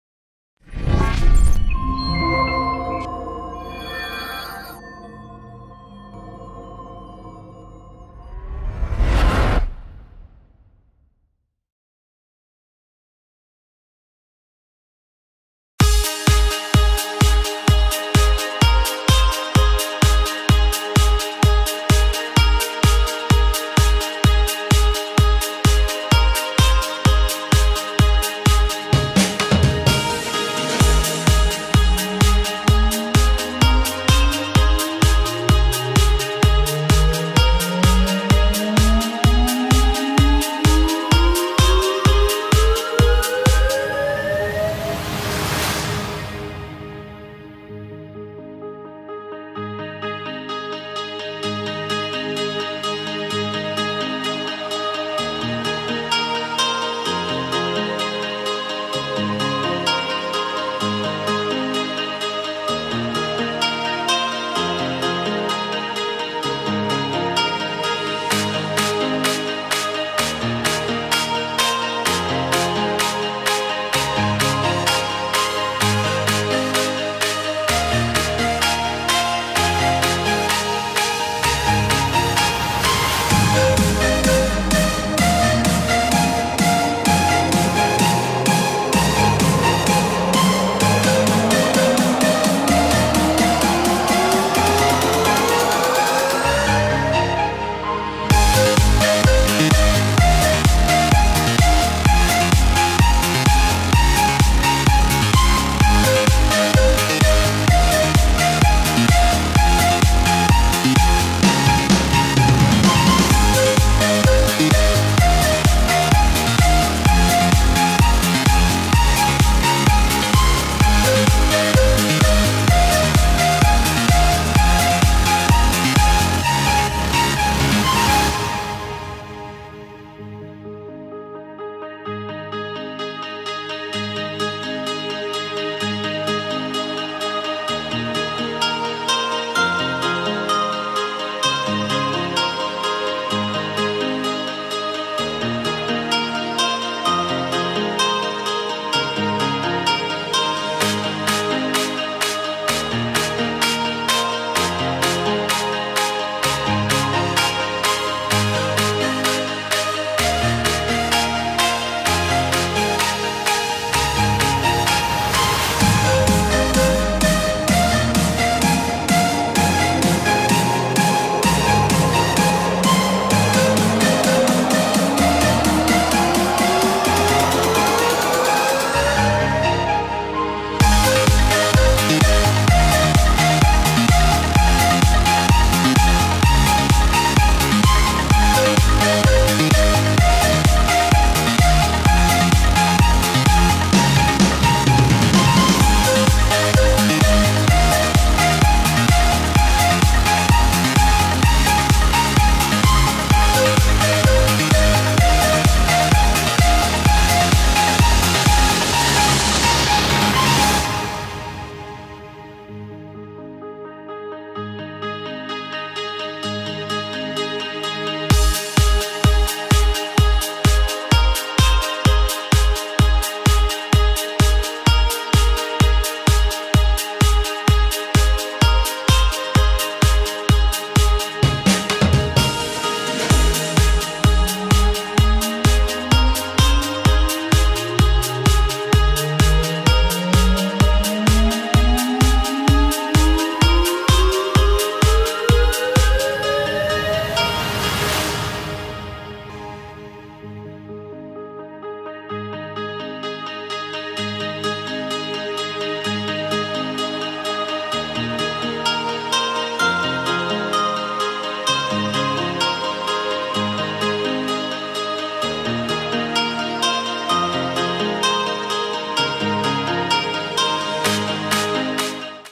Farewell At Taj Mahal Palace - Mumbai
Kenzie's farewell party at Taj Mahal Palace Hotel by JaneMED Team